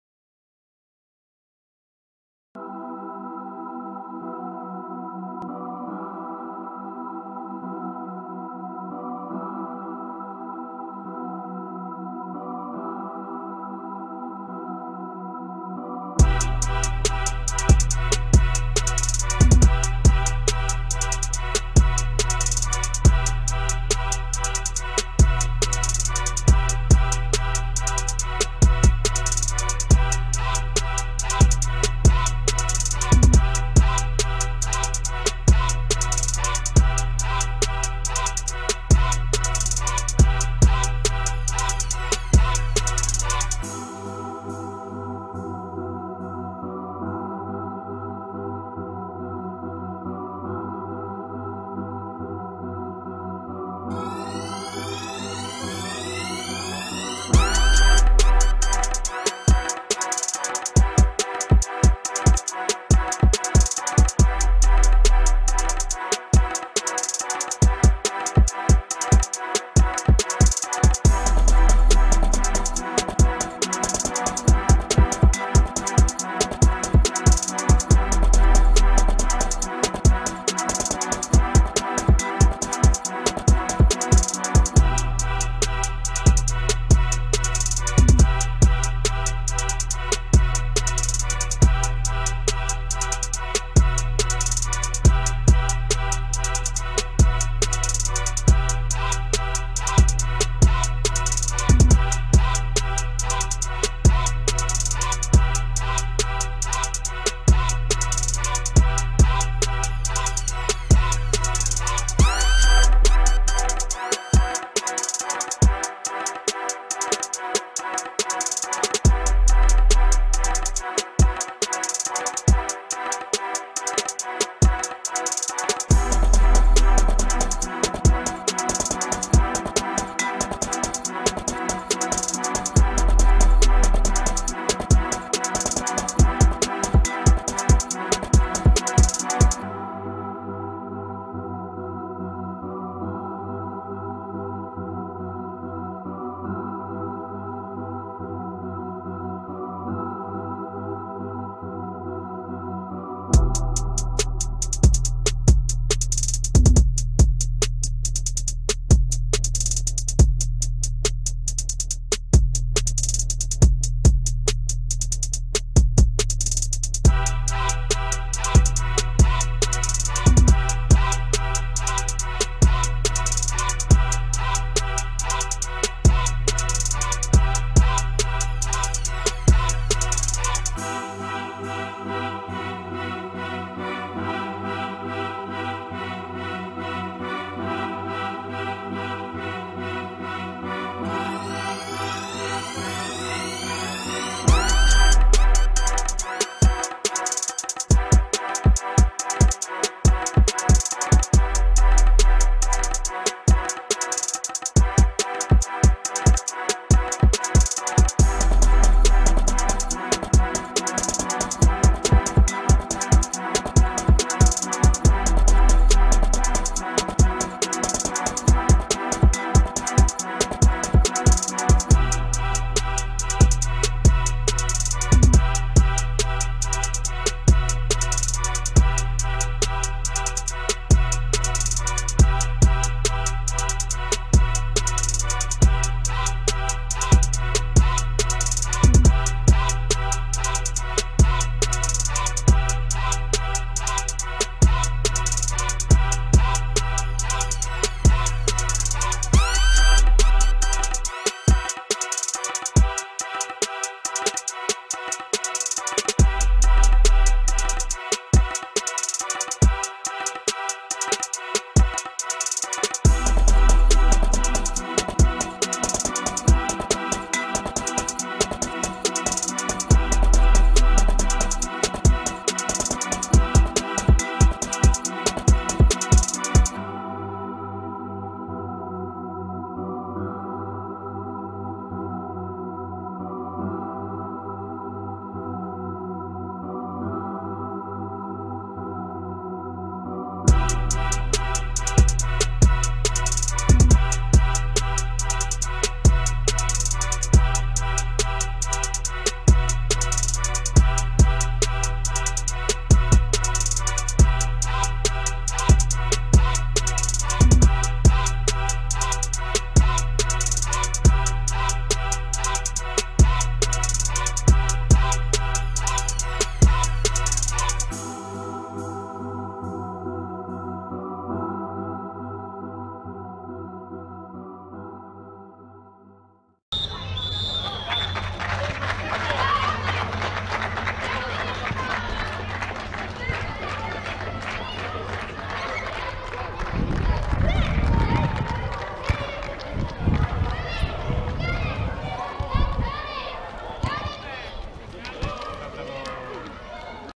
SEMP Ursynów vs Polonia Warszawa r.2008 (17.12.2017)
Skrót meczu ligi zimowej RS Sport pomiędzy KS SEMP Ursynów a MKS Polonia Warszawa  (rocznik 2008)